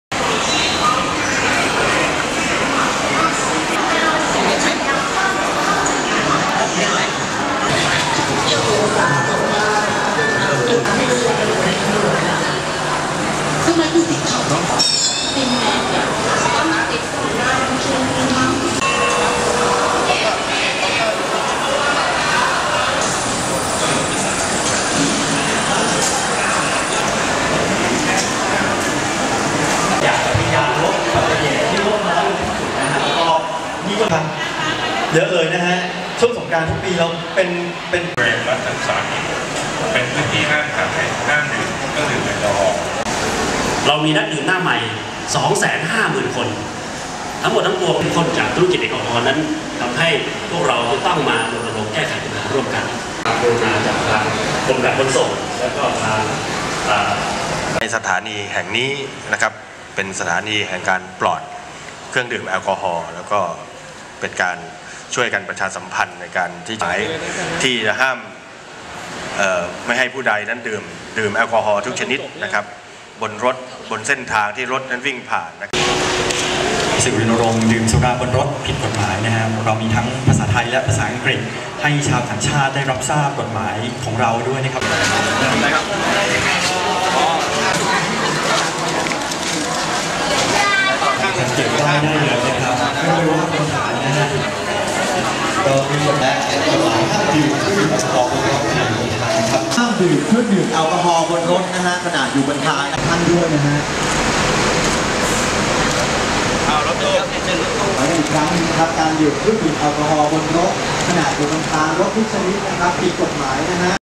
นรณรงค์ห้ามจำหน่ายและดื่มสุรา
นายธีระ วัชรปราณี ผู้จัดการสำนักงานเครือข่ายองค์กรงดเหล้า(สคล) และสำนักงานกองทุนสนับสนุนและสร้างเสริมสุขภาพ(สสส.) ร่วมกันรณรงค์ห้ามจำหน่ายและดื่มสุรา ในเขตสถานีขนส่ง และรถโดยสาร ที่สถานีขนส่งหมอชิต / Photographer ธัชดล ปัญญาพานิชกุล (Tatchadon Panyaphanitkul)